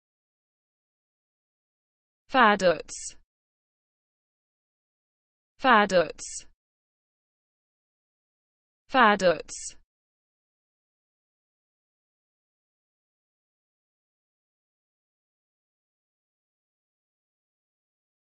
How to Pronounce vaduz with Meaning, Phonetic, Synonyms and Sentence Examples
This video shows pronunciation of vaduz in a sentence, vaduz meaning, vaduz definition, vaduz phonetic, vaduz synonym and vaduz example

vaduz Definition/Meaning : the capital and largest city of Liechtenstein
vaduz Phonetic : [vaduz]